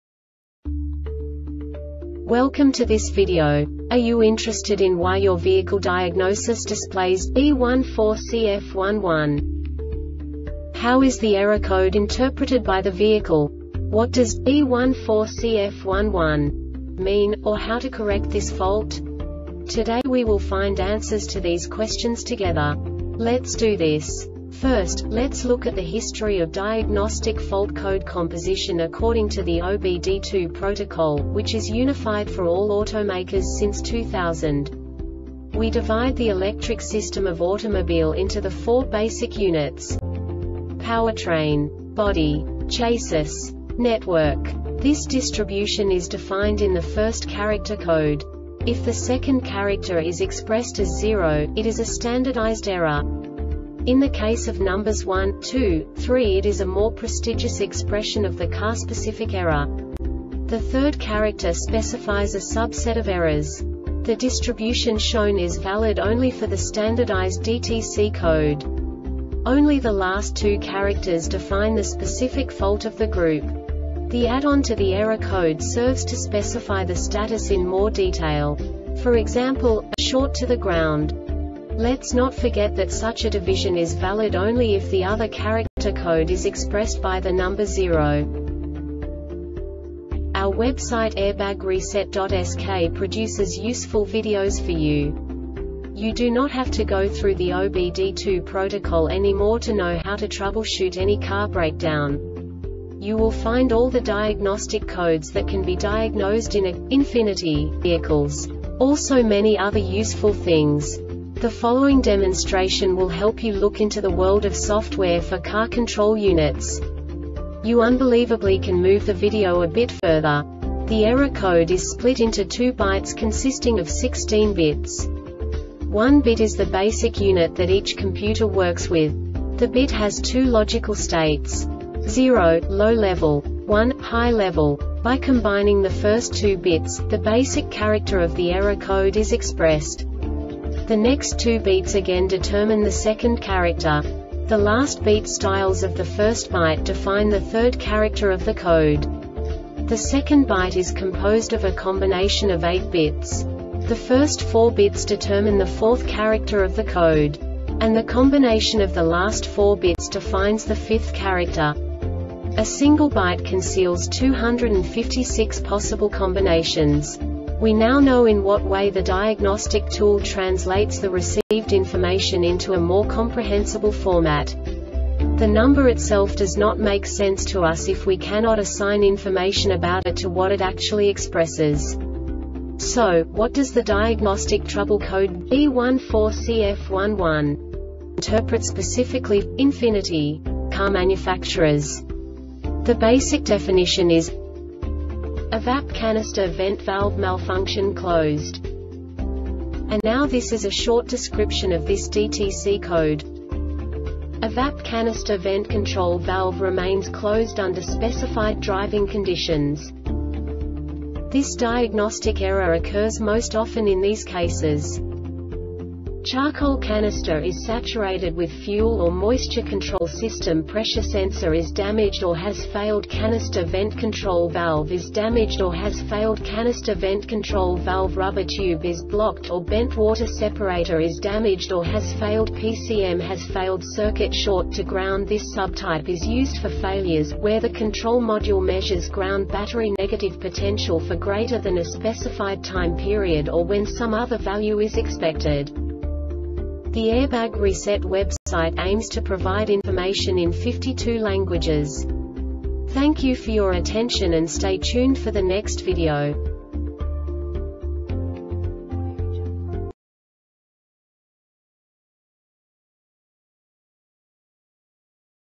DTC Ford B14CF-11 Short Explanation
Contents:
0:21 Basic DTC analysis according to OBD2 protocol standard.
1:48 Insight into programming
2:58 A diagnostically understandable description of the Diagnostic Trouble Code
3:05 Basic error definition

Make:
Ford
Code:
B14CF 11
Definition:
EVAP Canister Vent Valve Malfunction (Closed)
Description:
EVAP canister vent control valve remains closed under specified driving conditions.
Cause:
 Charcoal canister is saturated with fuel or moisture Control system pressure sensor is damaged or has failed Canister vent control valve is damaged or has failed Canister vent control valve rubber tube is blocked or bent Water separator is damaged or has failed PCM has failed
Failure Type: